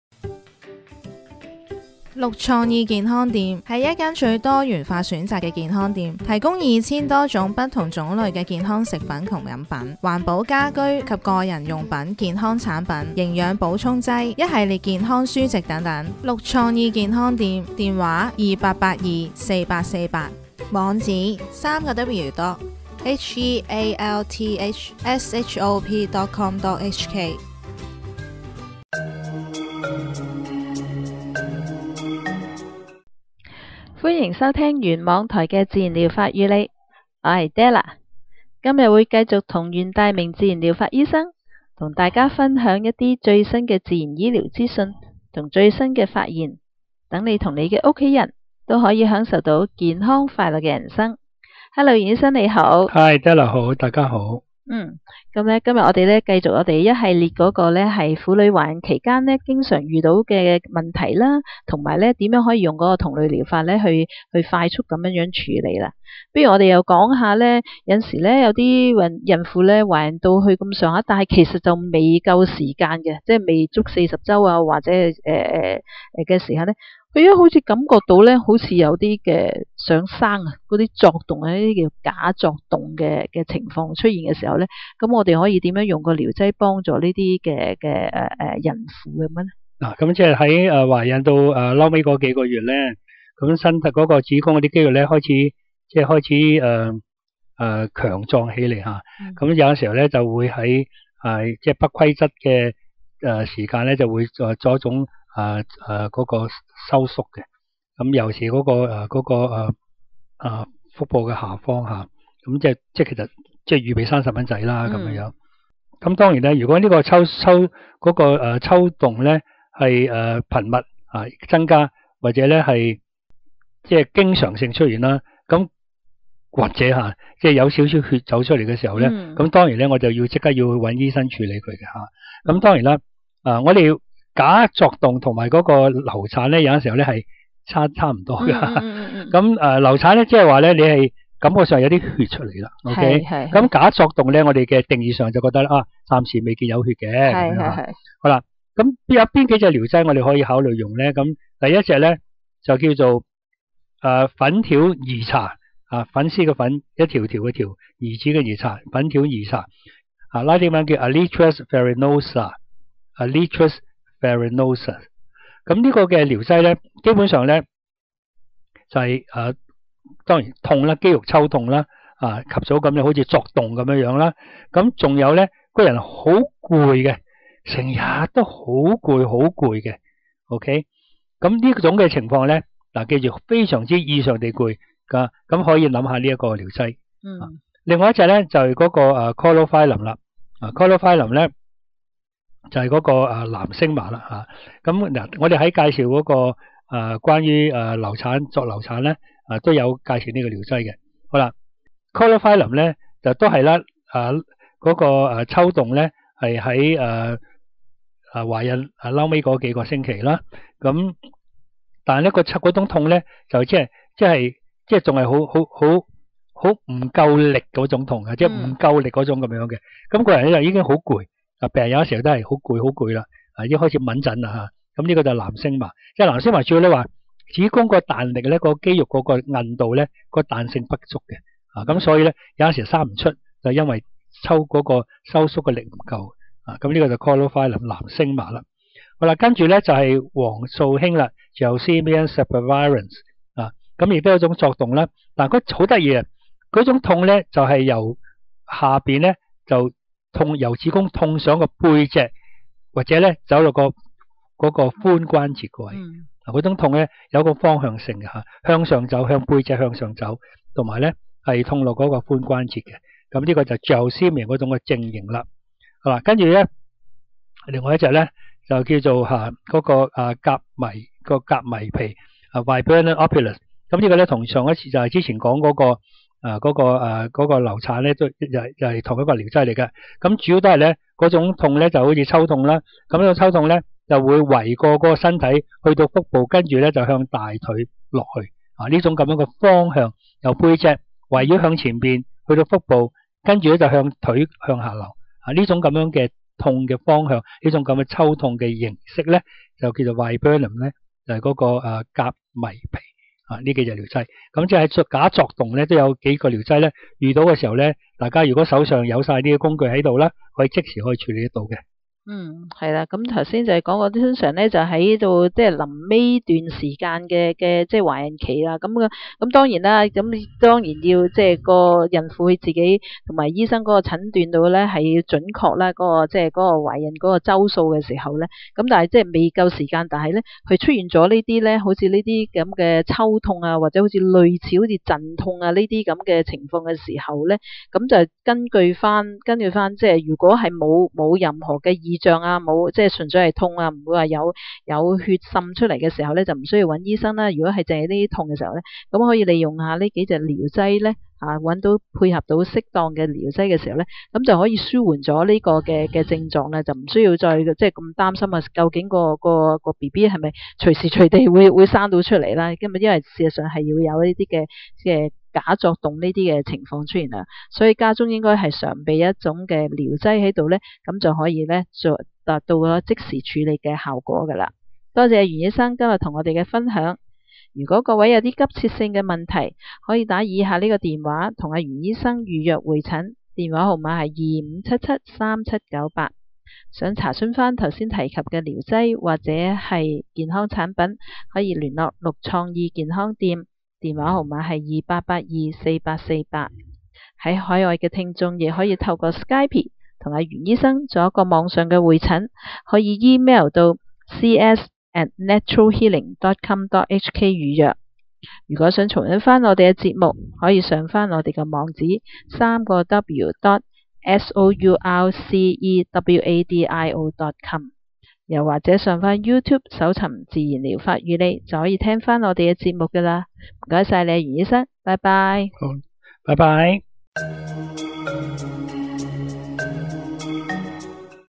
《自然療法與你》- EP620 - 假作動的同類療法
今集袁醫生和 Della 同大家分享如何使用同類療劑處理「假作動」（False Labor Pains）。
【處理假作動的主要同類療劑】
藍升麻 caulophyllum Thalictroides 30C：